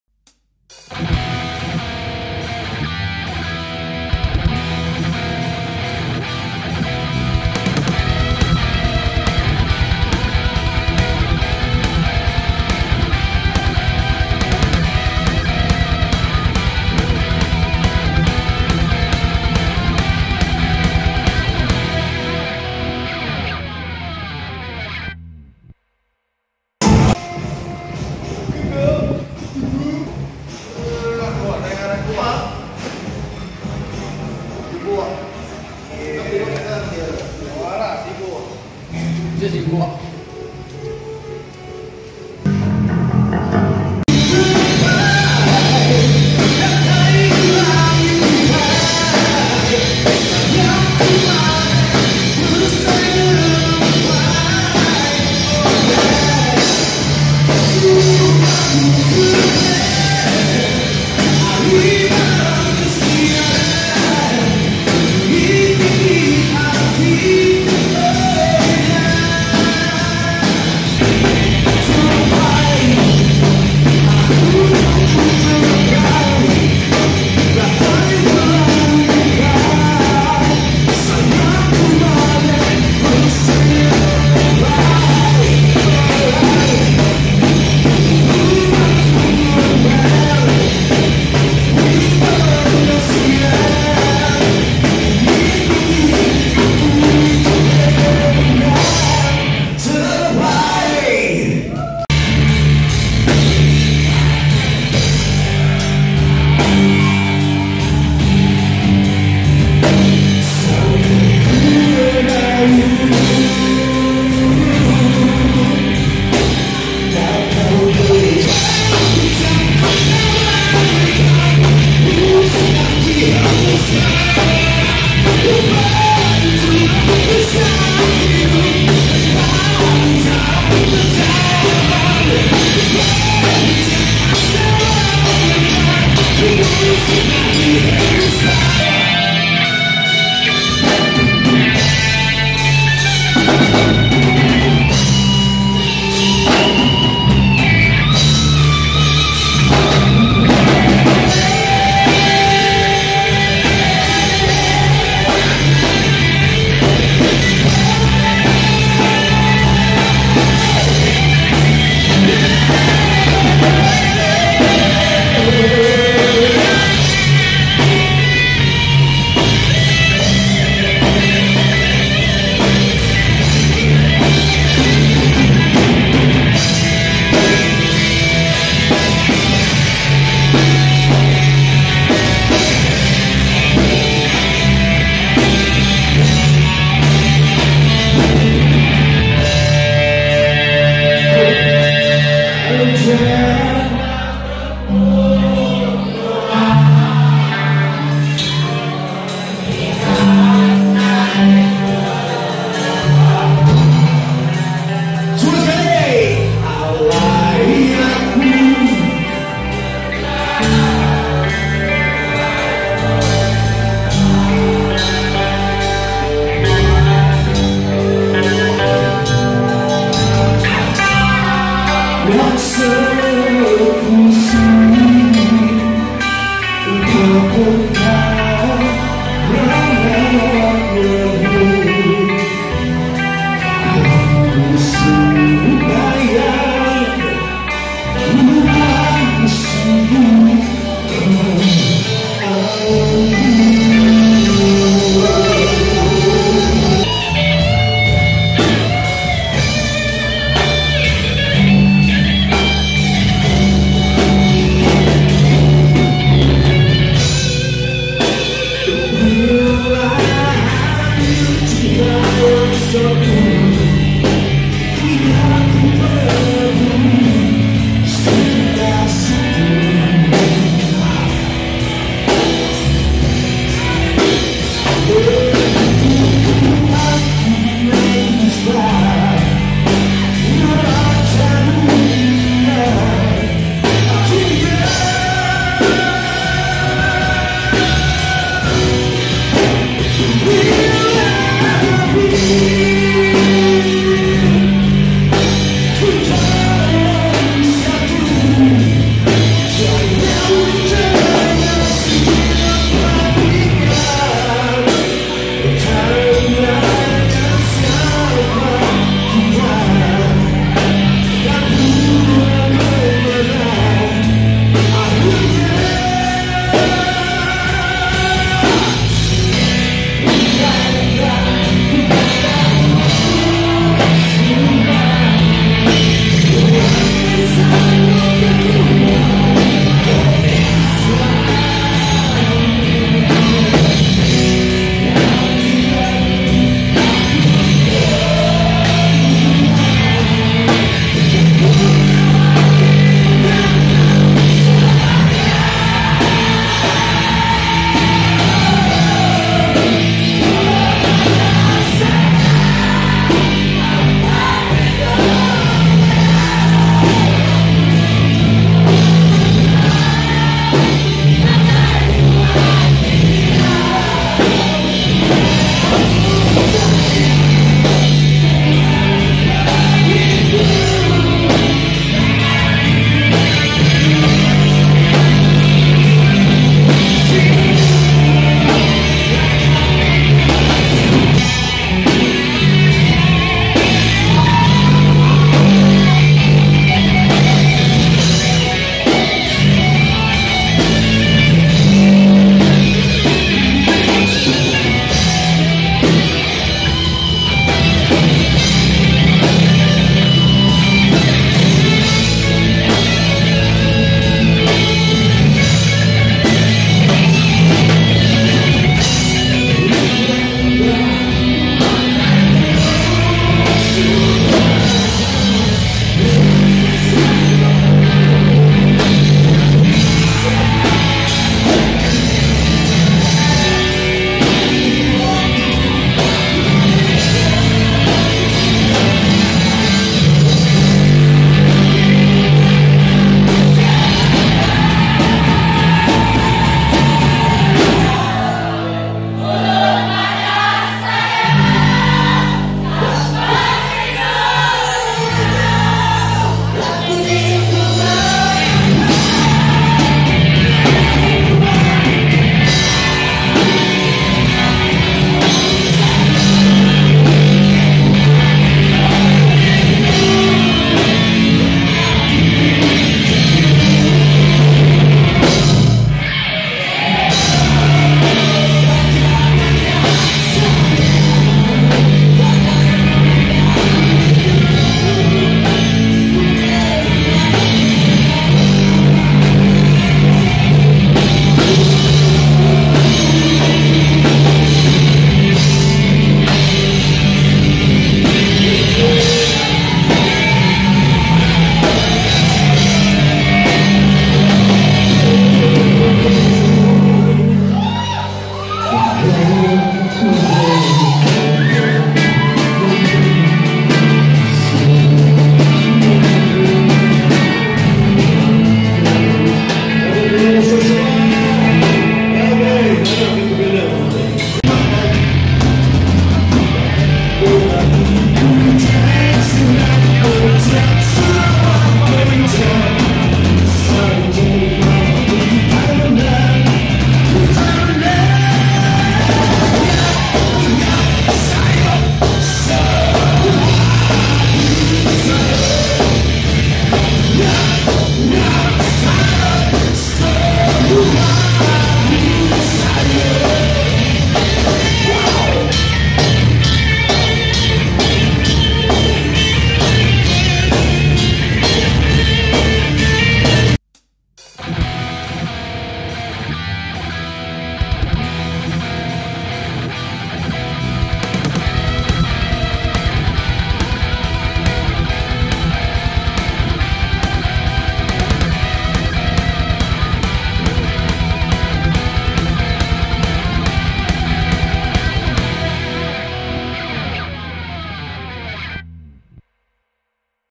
Rockumentary #3 Masterjam live at Panggau Keling 2020
Masterjam project band from Sibu live at Panggau Keling, Sarikei February 2020